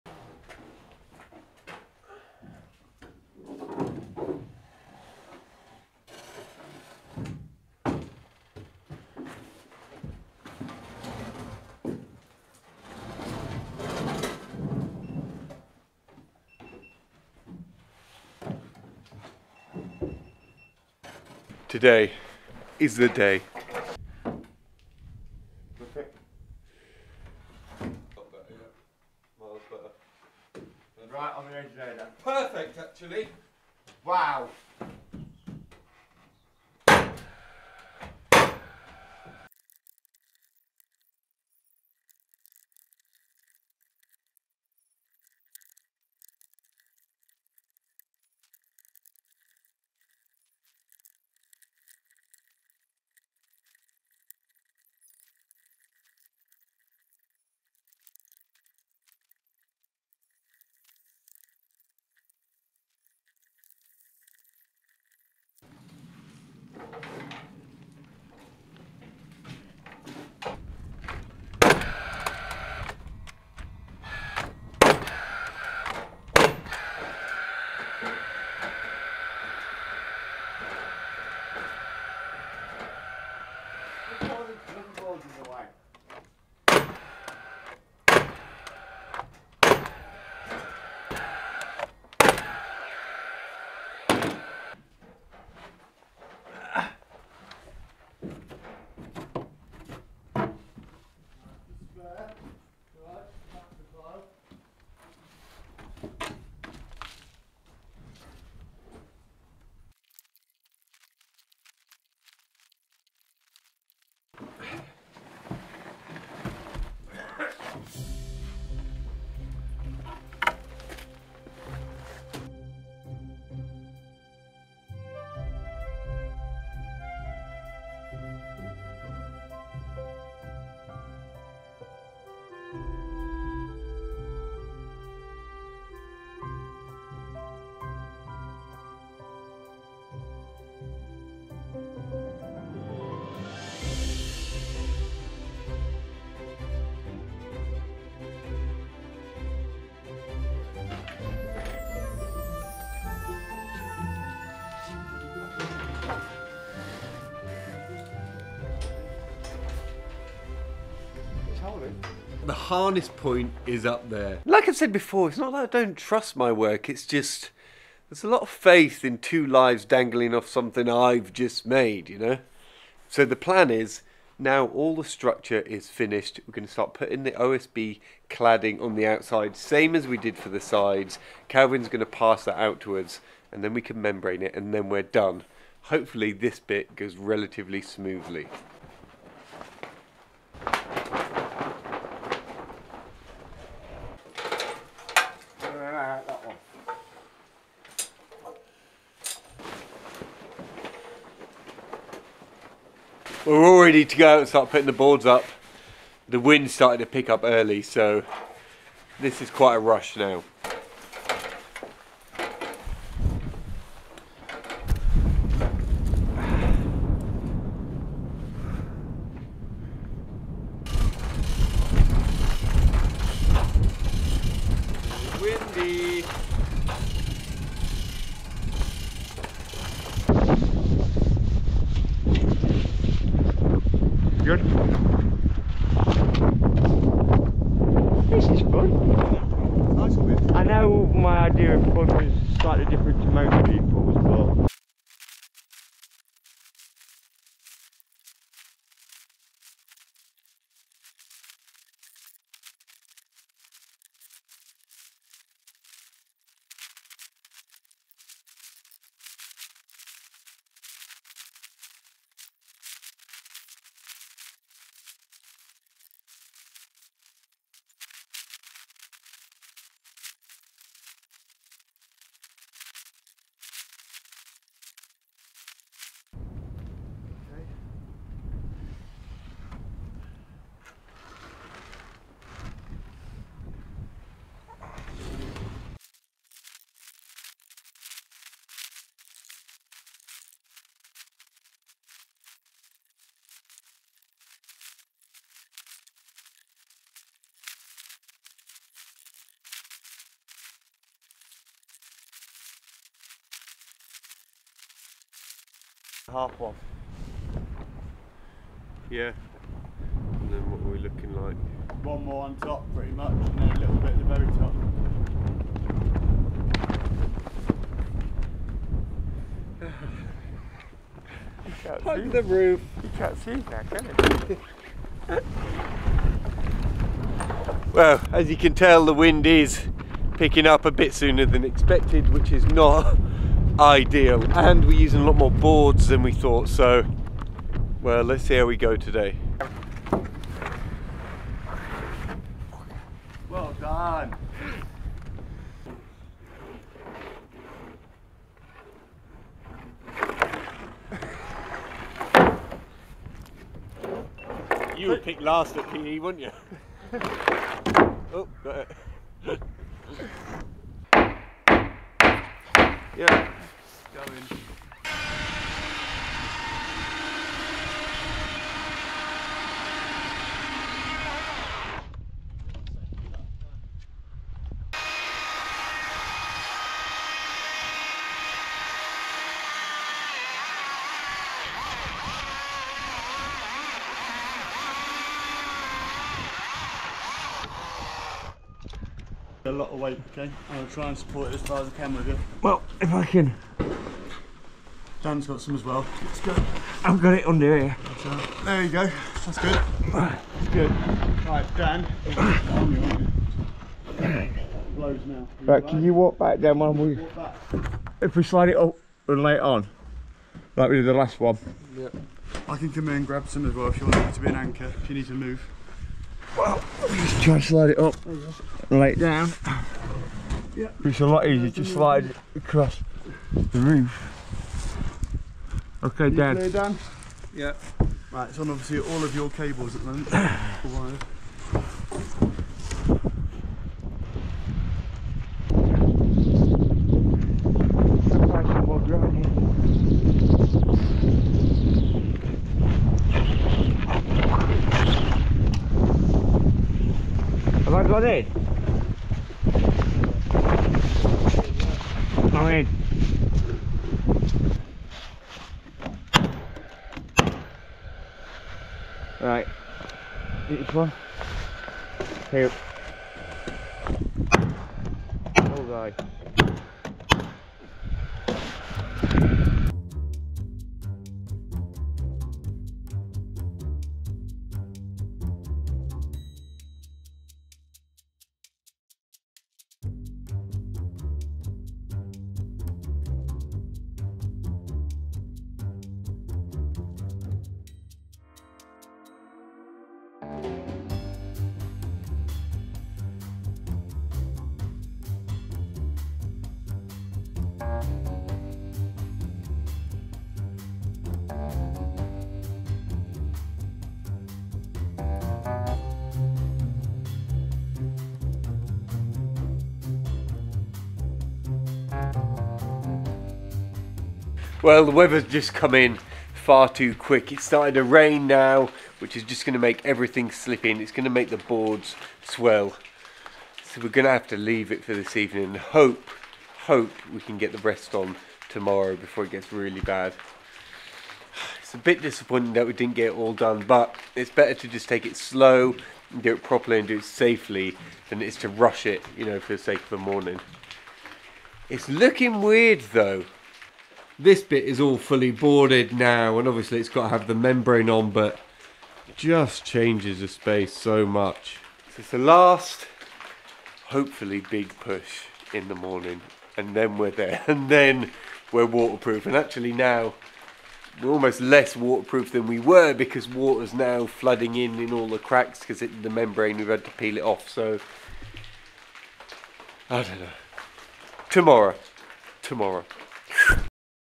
The storm came early, high winds at 25 metres up.
take a look at my workshop tools here;